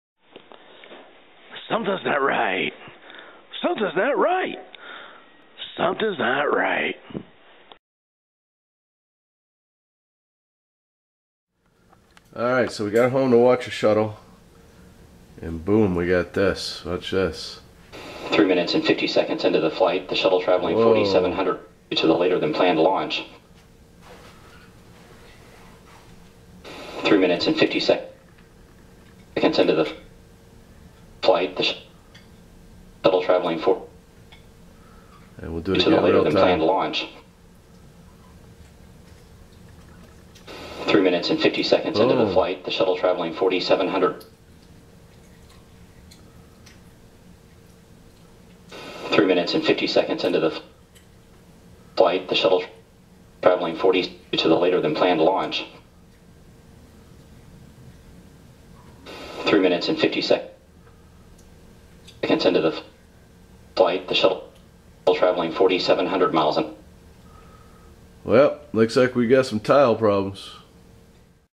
NASA Shuttle Discovery STS-133 - Debris from Fuel Tank
Looks like we may have some tile damage. Let's hope it is not too bad!